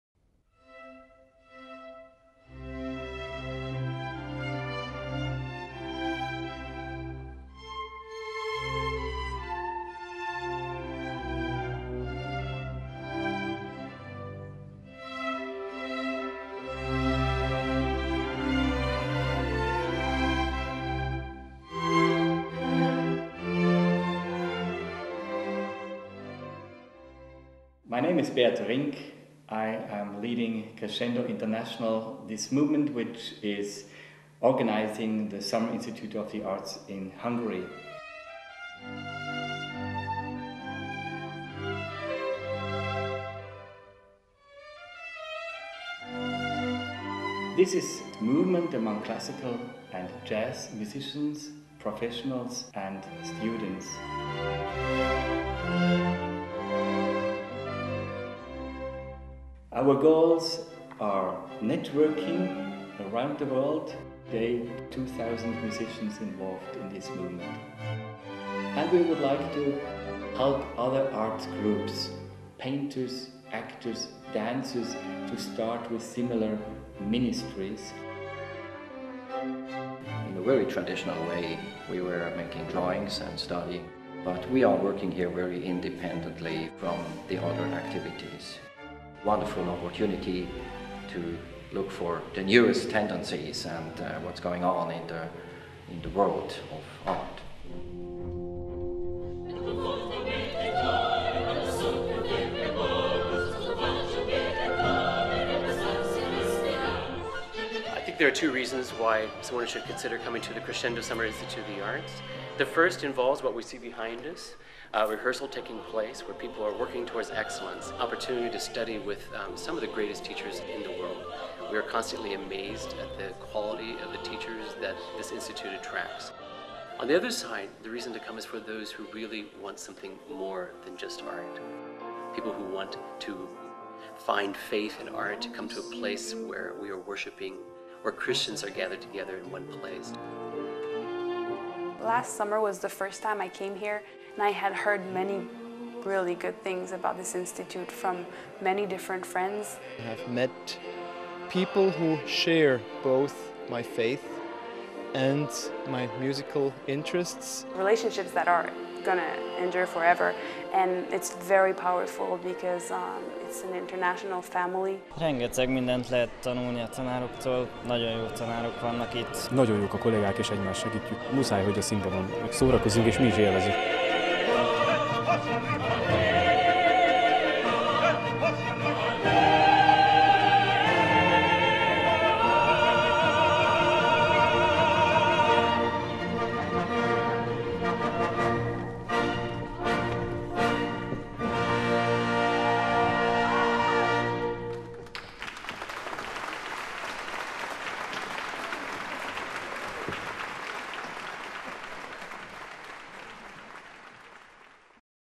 Crescendo Summer Institute 2013
The Crescendo Summer Institute, one of the leading academies in Central Europe, offers an outstanding instruction for talented students and young professionals in the area of classical, jazz music and other art forms (theatre, visual art) to become successful and recognized artists, who desire more than just professional excellence.

CRESCENDO NYÁRI AKADÉMIA 2013
SÁROSPATAK
2013. JÚLIUS 29 - AUGUSZTUS 12.

A Crescendo Nyári Akadémia azoknak a tehetséges diákoknak a választása, akik magas szintű művészi tudásra törekednek, eredményes, szakmailag elismert művészként képzelik el jövőjüket, és fontosnak tartják, hogy művészetük több legyen pusztán szakmai kiválóságnál.